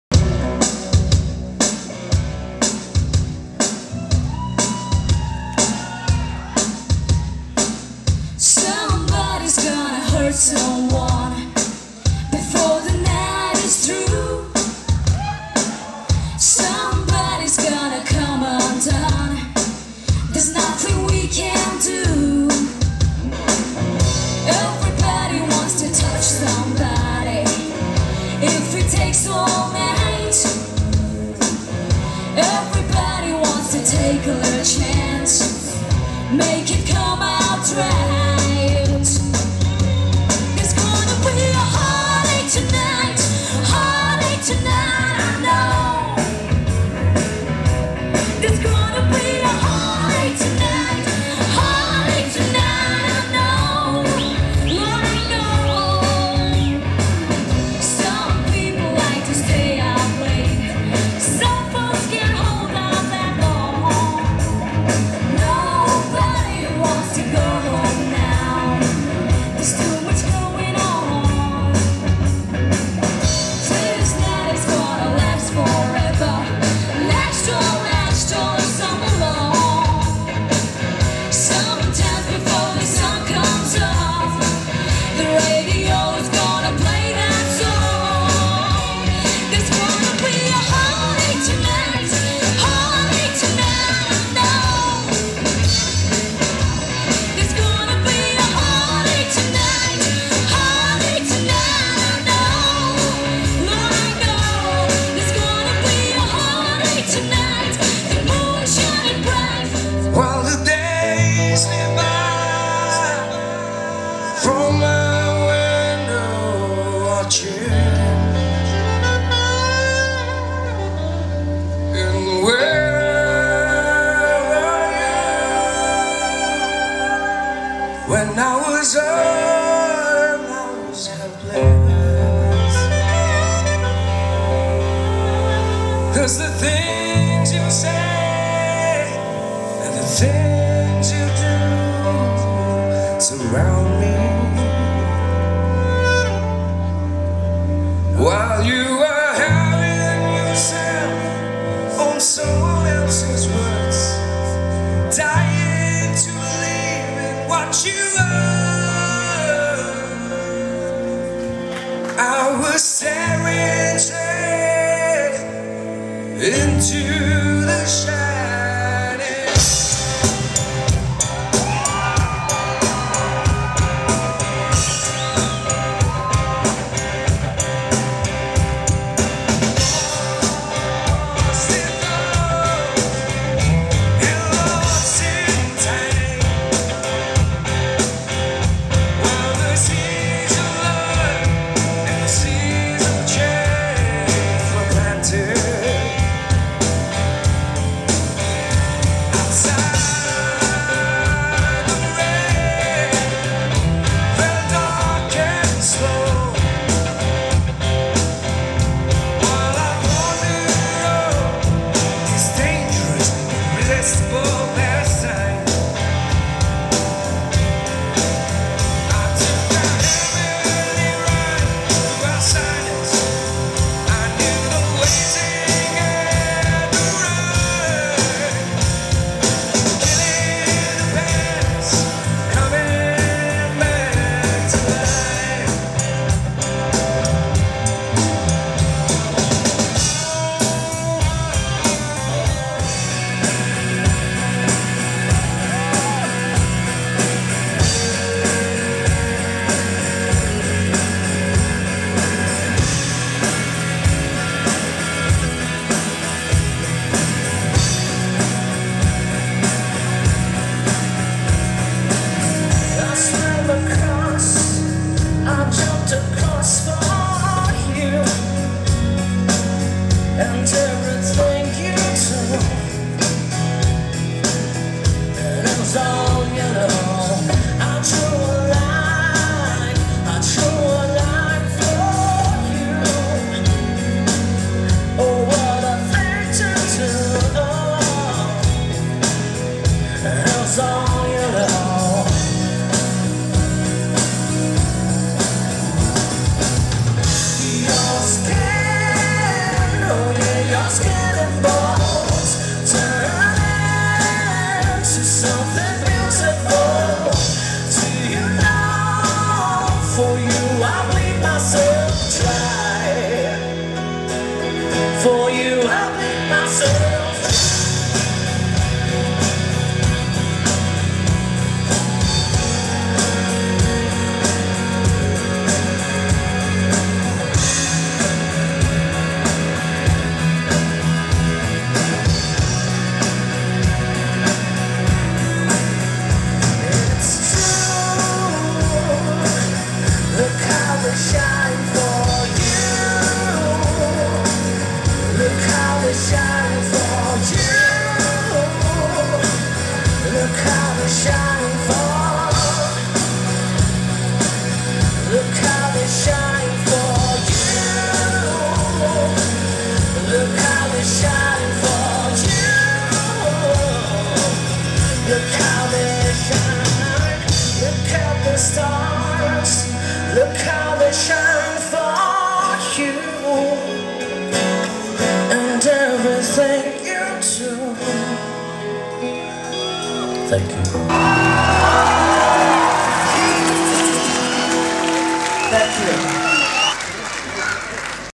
Concert-Centenary Celebration @MananKendra 19 8 2017
CENTENARY CELEBRATION
Concert at Manan Kendra on 19th Aug. 2017 with Abishek Lemu Gurung, Neha & Smyerna, EMRS Gangyap & Bamboo Groove